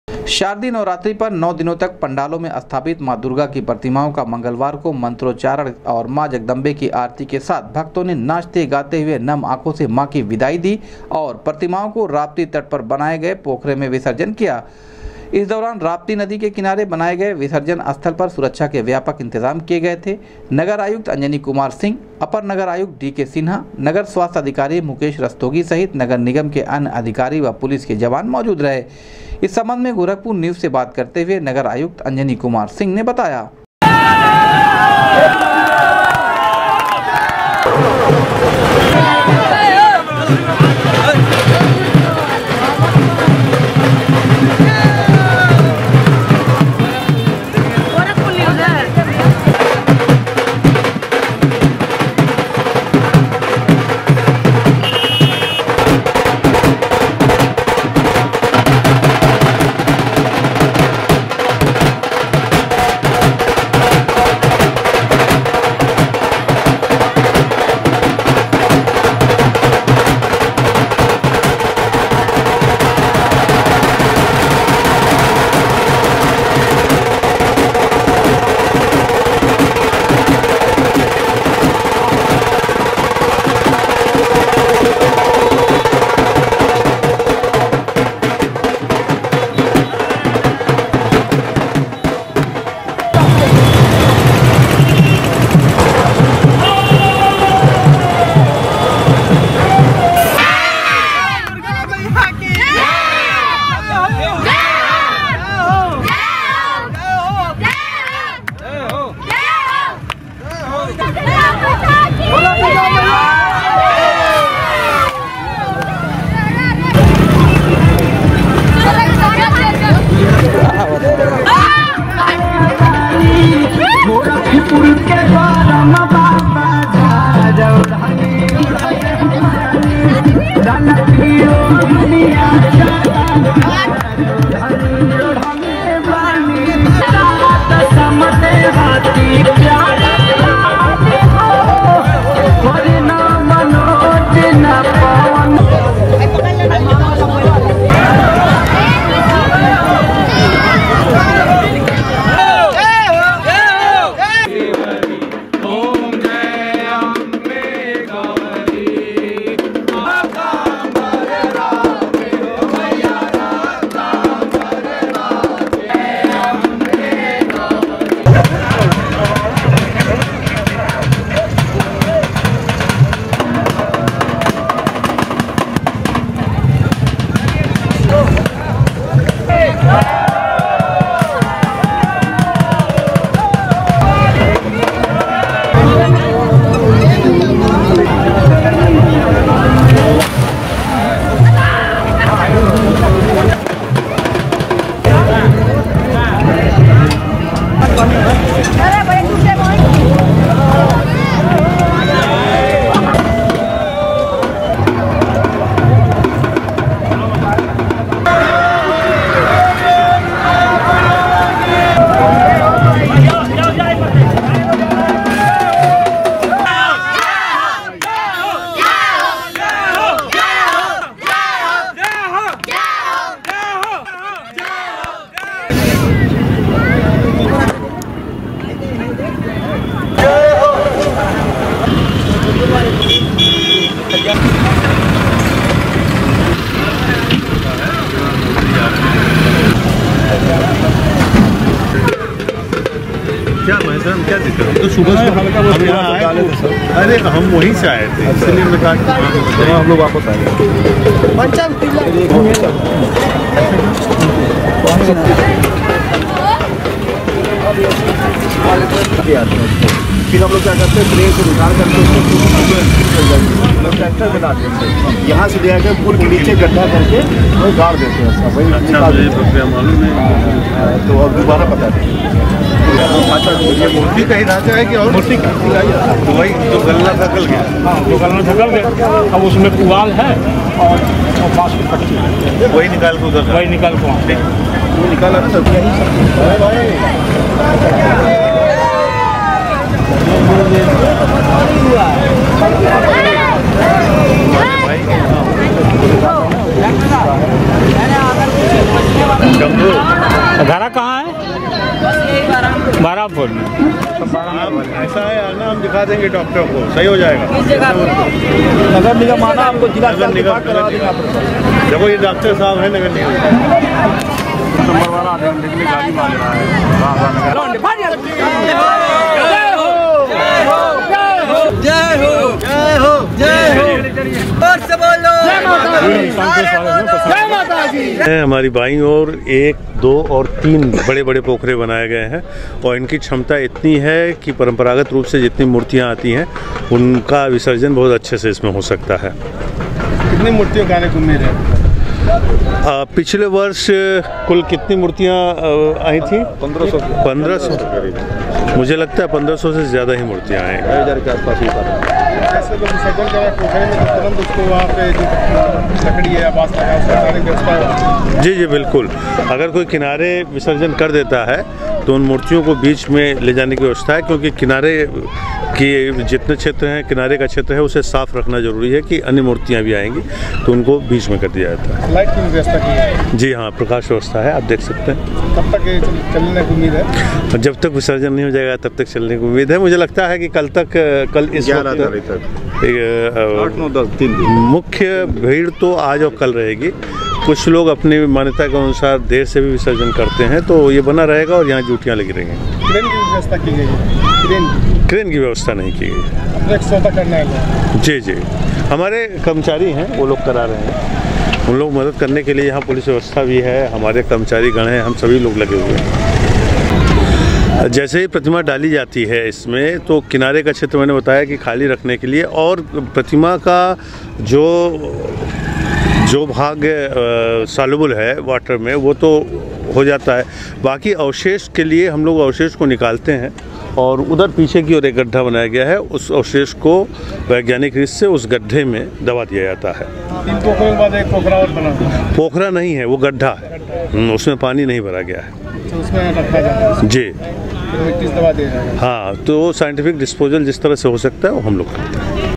दुर्गा प्रतिमाओं का भक्तो ने नाचते गाते हुए किया विसर्जन,सुरक्षा के रहे व्यापक इंतजाम
नव दिनों तक माँ दुर्गा के पंडालों में स्थापित दुर्गा प्रतिमाओं का भक्तो ने नाचते गाते हुए किया विसर्जन,सुरक्षा के रहे व्यापक इंतजाम